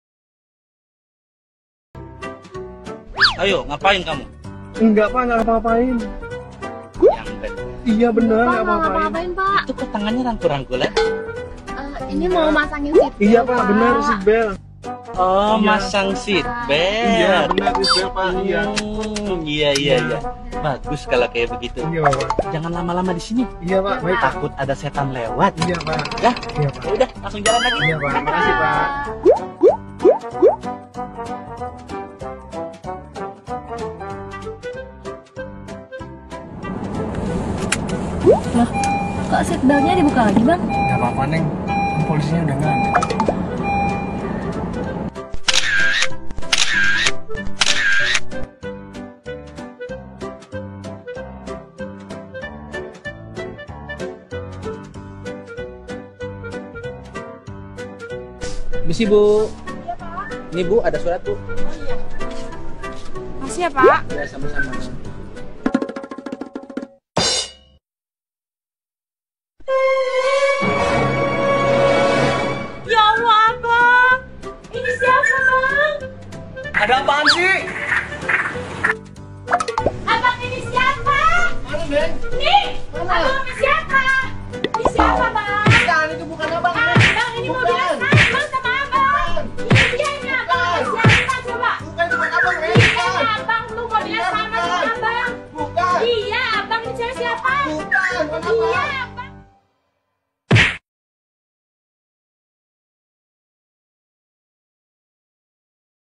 Awas Kena Tilang Istri Tua Juga Wkwkwkwkwk || Tilang Elektronik✓
Tilang Elekronik sudah mulai berlaku di beberapa daerah di Indonesia gaes... Lengkapi surat-suratnya dan tentunya patuhi aturan dalam berlalulintas.

Video ini milik Polda Metro Jaya